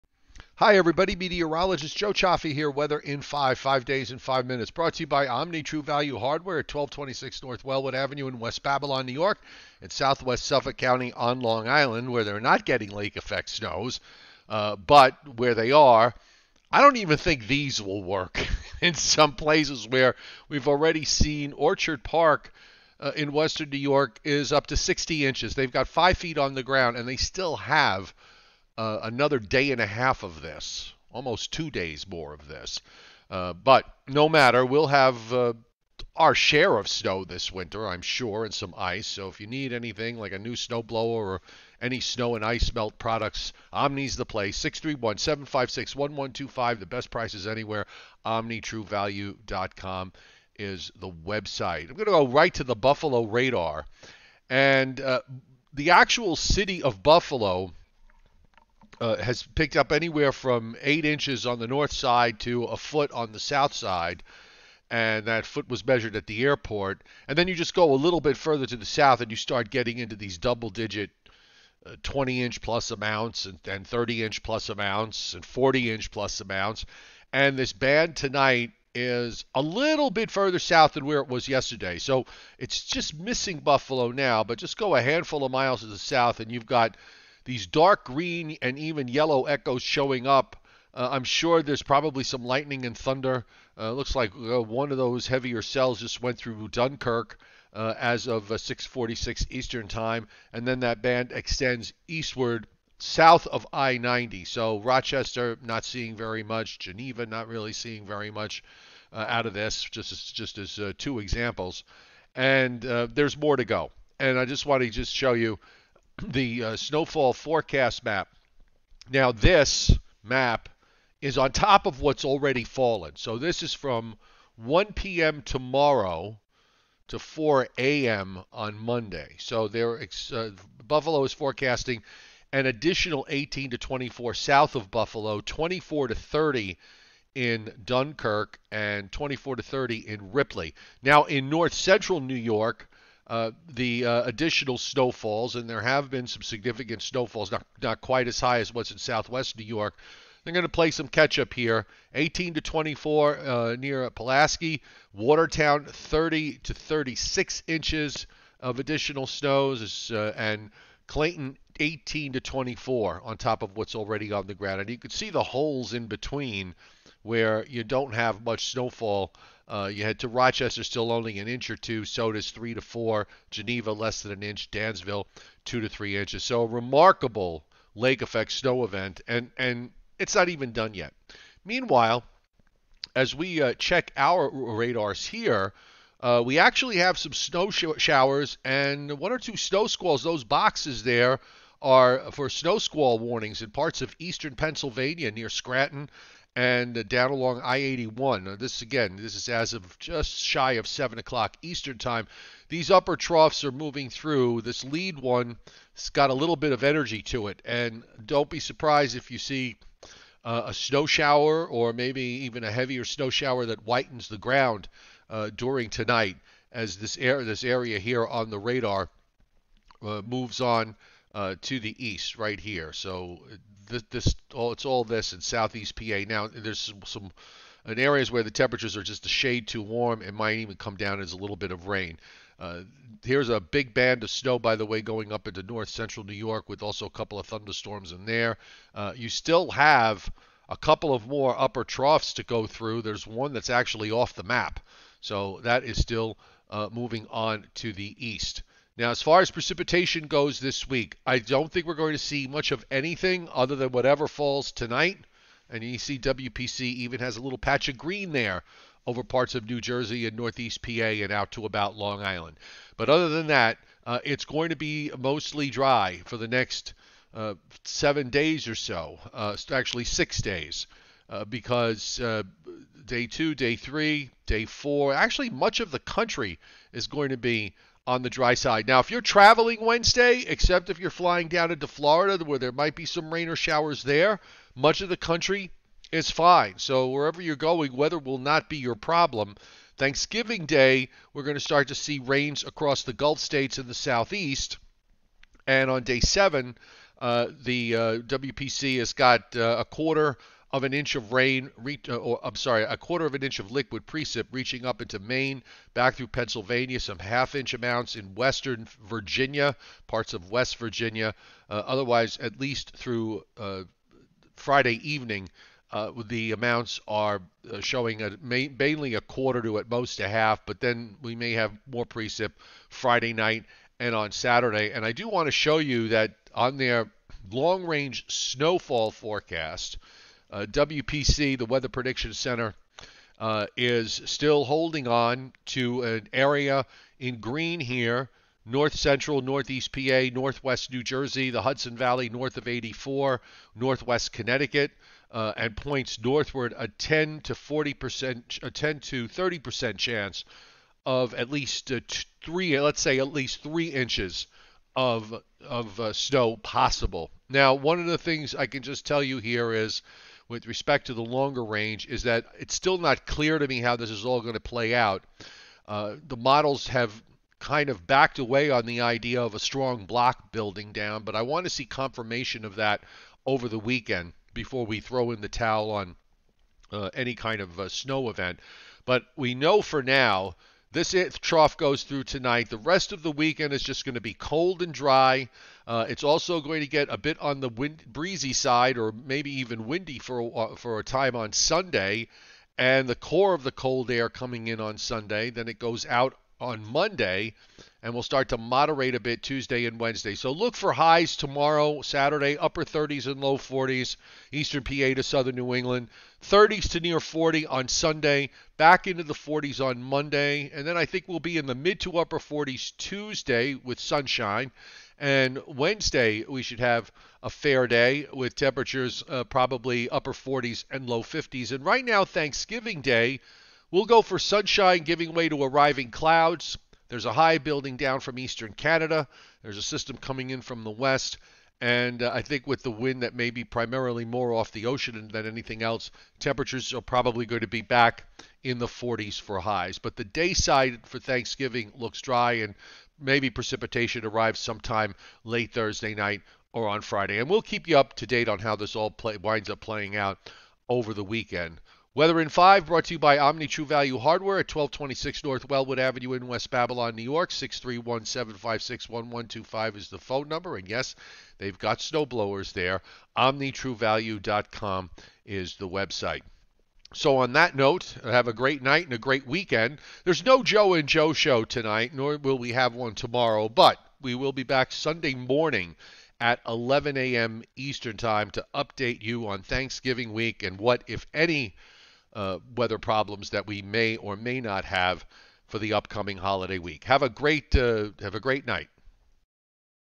Weather in 5 Major Lake Effect Snow Storm Continues, Weekend & Thanksgiving Week Outlook
Weather in 5 the major lake effect snow event continues right into Sunday and we have already seen as much as 5 feet of snow in parts of Southwestern NY. Meanwhile an upper trough is producing some snow showers and snow squalls and they are moving through Rastern Pennsylvanis to Southern New England overnight. The record lake effect snows will continue Saturday across Western and North Central NY, NW Pennsylvania and NE Ohio and should start to relax some later Sunday. The week ahead looks dry for most of the country through Wednesday. Thanksgiving day should be dry through much of the East. We are still watching the evolution of the next storm system for Friday and Saturday of next week.

Omni True Value Hardware has everything you need to get through this winter free of snow and ice